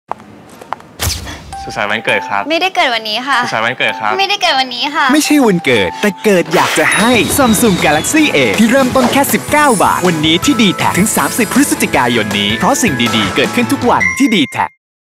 Galaxy A Series ที่ดีแทค เริ่มต้นแค่ 19.-
พิเศษจากดีแทค Samsung Galaxy A ที่เริ่มต้นแค่ 19 บาท
ตั้งแต่ 15 ต.ค. 62 - 30 พ.ย. 62 เพราะสิ่งดีๆ เกิดขึ้นทุกวัน ที่ดีแทค

รายละเอียดเพิ่มเติม คลิก